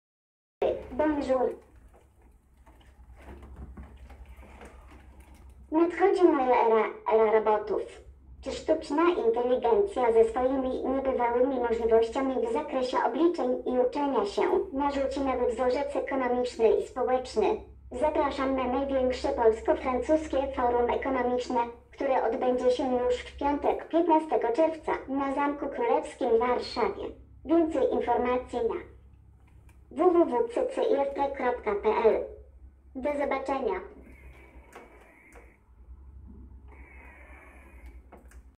Robot Pepper zaprasza na Spotkania Warszawskie do Zamku Królewskiego
15 czerwca, na Zamku Królewskim w Warszawie odbędzie się największe polsko-francuskie forum ekonomiczne. Zaproszeni goście, w tym Luc Ferry, Stephane Richard, czy Janusz Filipiak, rozmawiać będą o nachodzącej erze robotów. // Le 15 juin, au Château Royal de Varsovie aura lieu le plus grand forum économique franco-polonais. Parmi les intervenants vous trouverez entre autres Luc Ferry et Stéphane Richard qui discuteront sur l’ère des robots.